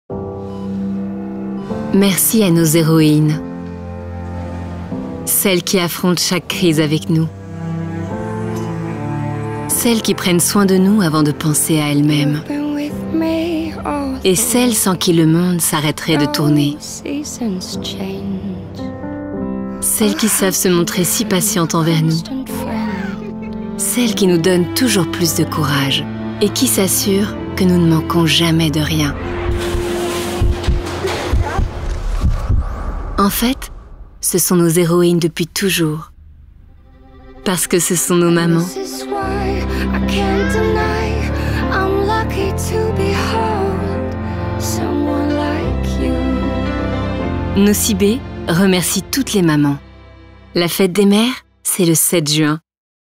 Nocibé remercie toutes les mamans
Merci à nos héroïnes.
Celles qui affrontent chaque crise avec nous,
Celles qui prennent soin de nous avant de penser à elles-mêmes,
Et celles sans qui le monde s’arrêterait de tourner.
Celles qui savent se montrer si patientes envers nous,
Celles qui nous donnent toujours plus de courage,
Et qui s’assurent que nous ne manquons jamais de rien.
En fait, ce sont nos héroïnes depuis toujours.
Parce que ce sont nos Mamans...

La fête des Mères, c’est le 7 juin.